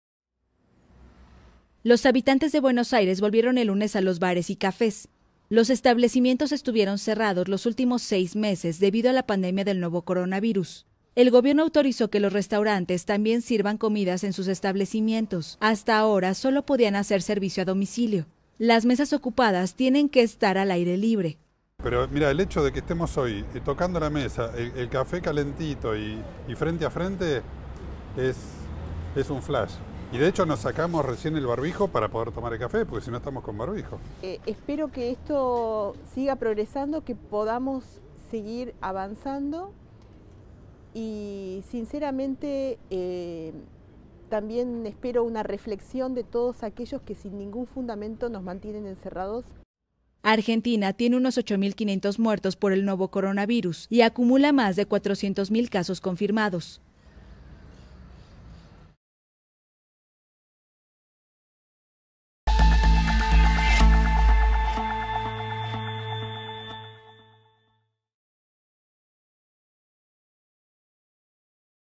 Bares y cafés reabren en Buenos Aires
Noticias de último momento: Argentinos aprovecharon el lunes para visitar bares y cafés de Buenos Aires. El gobierno autorizó la apertura tras seis meses de permanecer cerrados por el nuevo coronavirus.